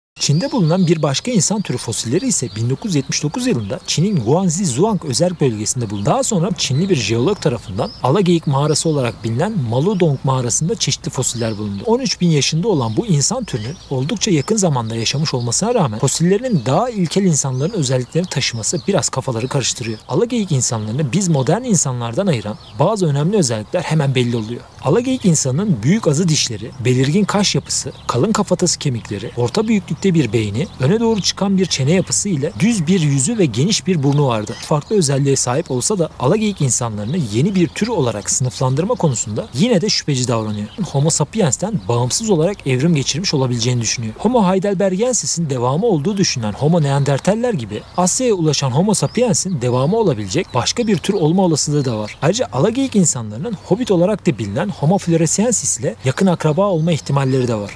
İnsanlığın Kökenine Yolculuk: Çin'deki Alageyik İnsanları Fosilleri
Çin'in gizemli topraklarında unutulmuş bir geçmişin izlerini keşfedin! "Çin'de Bulunan Alageyik İnsanları Fosilleri" adlı videoya hoş geldiniz. Bu benzersiz belgeselde, Çin'de ortaya çıkan Alageyik insanları fosillerinin sırlarını açığa çıkarıyoruz.
Binlerce yıl öncesine, insanlık tarihindeki önemli bir döneme tanıklık edeceksiniz. Çin'deki kazılar sırasında bulunan Alageyik insanları fosilleri, arkeologları ve antropologları büyülemiş ve insan evriminin anlaşılmasına yeni bir perspektif sunmuştur. Bu videoda, bu heyecan verici keşfin detaylarına adım adım yaklaşacak ve bu esrarengiz insan türünün hikayesini keşfedeceksiniz.

Fosillerin bulunuşu, yapılan analizler ve bilimsel araştırmalar, Alageyik insanlarının anatomik özellikleri, yaşam tarzı ve insan evrimine olan etkileri hakkında değerli ipuçları sunmaktadır. Video boyunca, arkeologların ve bilim insanlarının çalışmalarını izleyecek ve bu fosillerin nasıl insanlık tarihindeki yerini aldığını öğreneceksiniz.

Bu video, sizi derinlere götürecek ve insanlık tarihindeki gizemli bir döneme ışık tutacak. Görsel açıdan etkileyici sahneler, anlatıcı tarafından anlatılan bilgiler ve ilgi çekici detaylarla dolu olan bu video, arkeoloji ve antropolojiye ilgi duyan herkes için vazgeçilmez bir deneyim sunuyor.